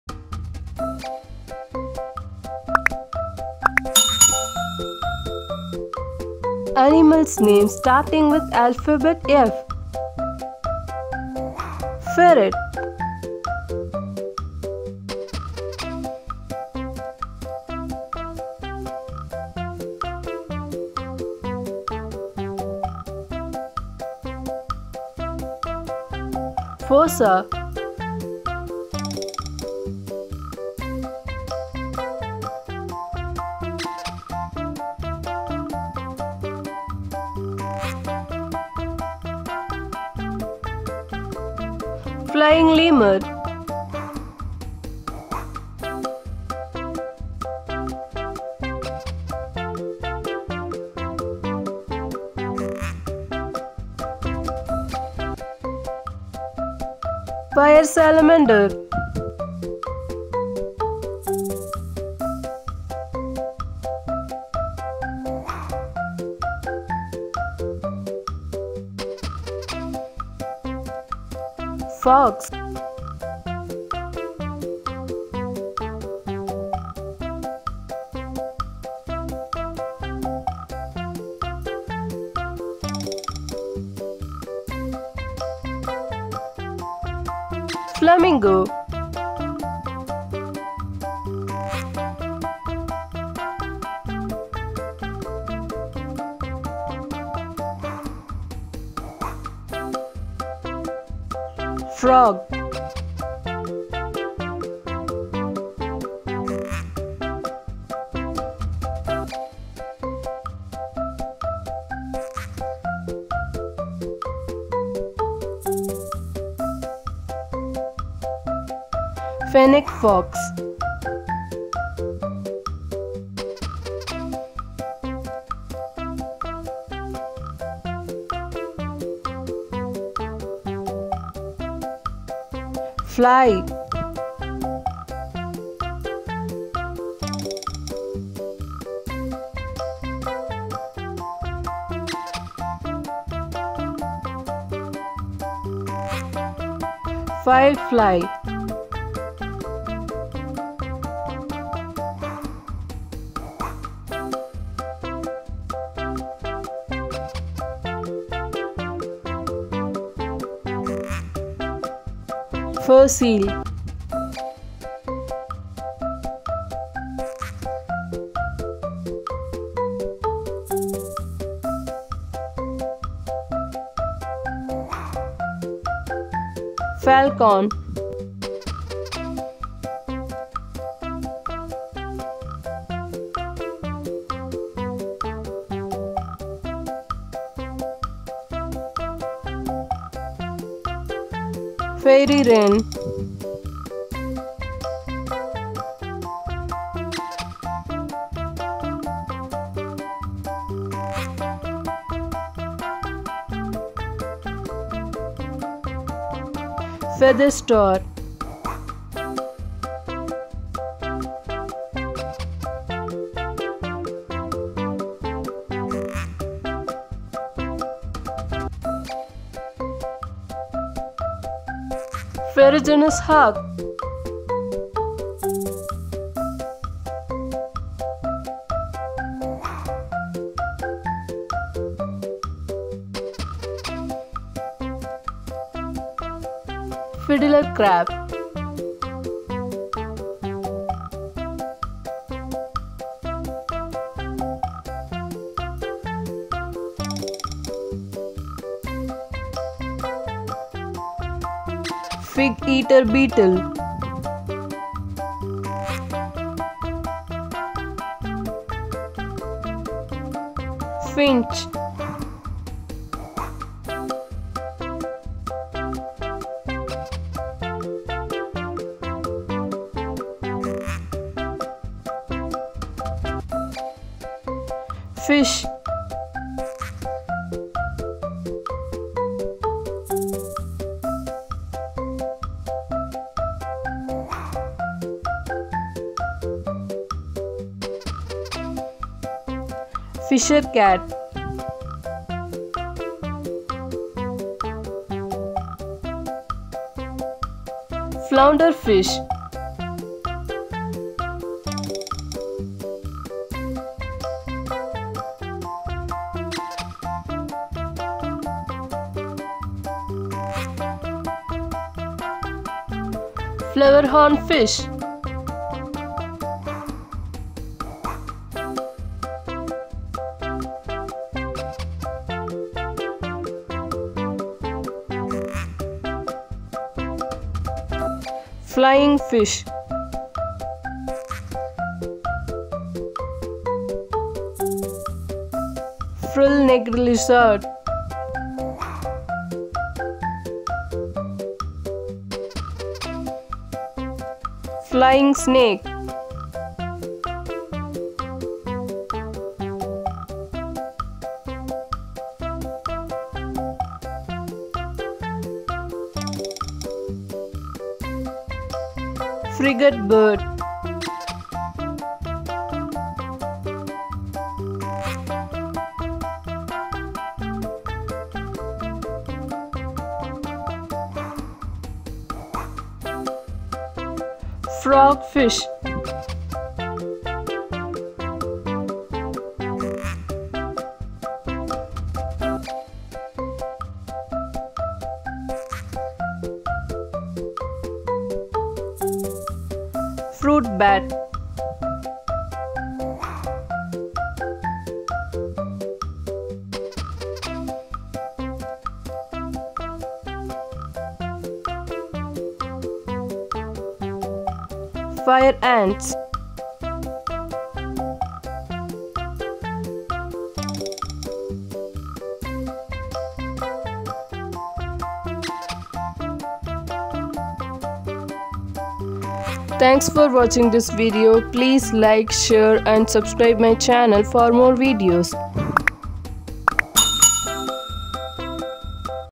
Animals Names Starting With Alphabet F | 30 Animals Names for Kids
This video is very useful to learn about animals names starting with alphabet F.